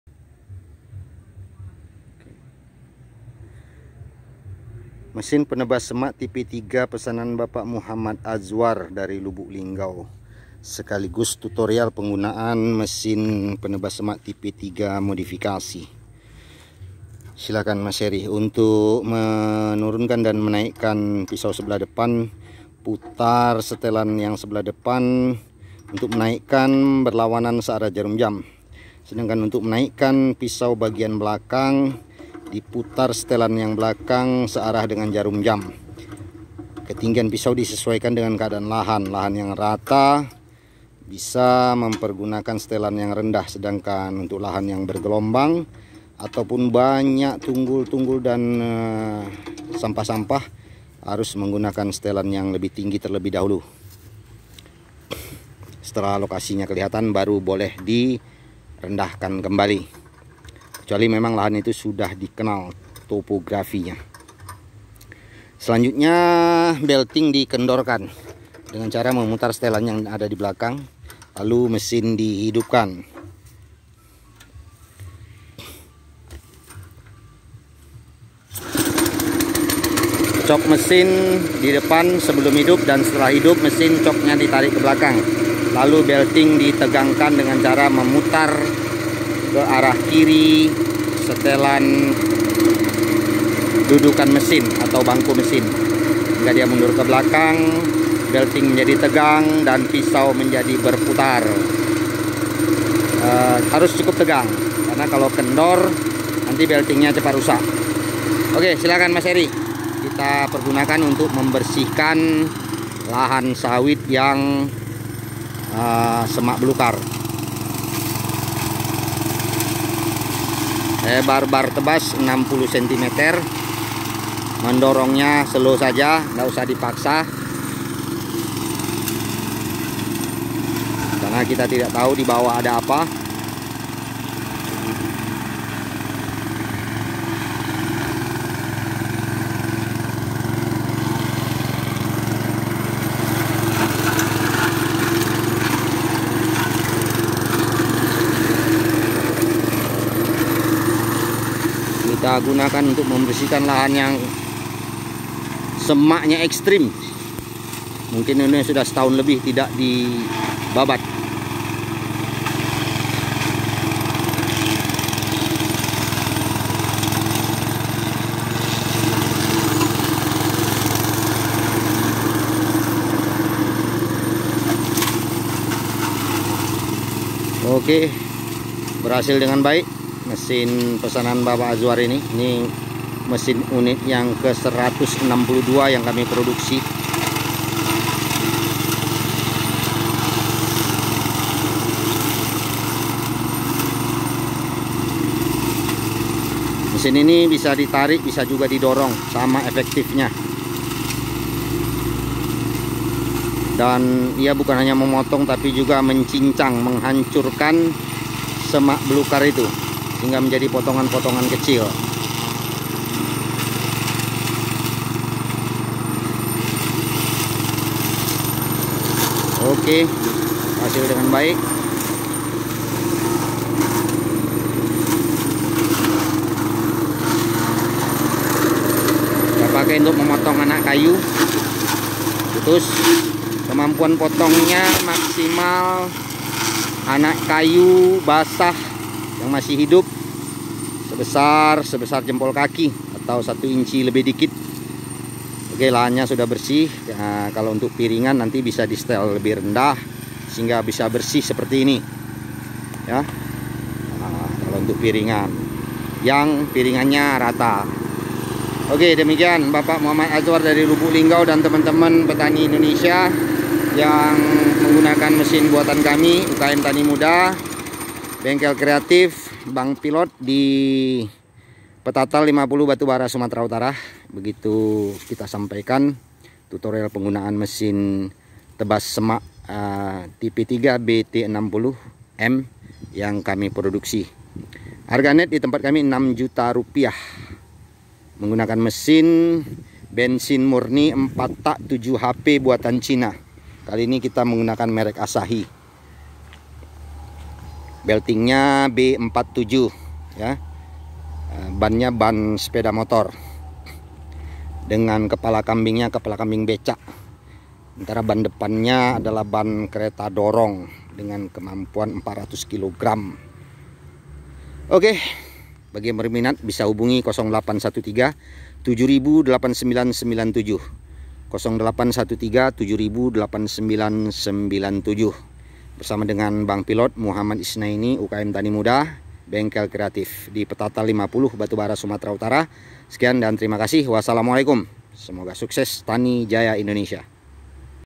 Tutorial Penggunaan Mesin Penebas Semak T3BT60N
Harga nett 6jt per unit blm ongkir. Mesin 7 hp buatan China. Minyak bensin murni.  Lebar bar tebas 60 cm. Bisa menebas anak kayu diameter 1 inch. Setelan tinggi pisau dari tanah 0 sd 12 cm, bebas. Daya kerja sekitar 1 ha lahan kebun dlm 1 hari kerja. Konsumsi minyak sekitar 10 liter per ha.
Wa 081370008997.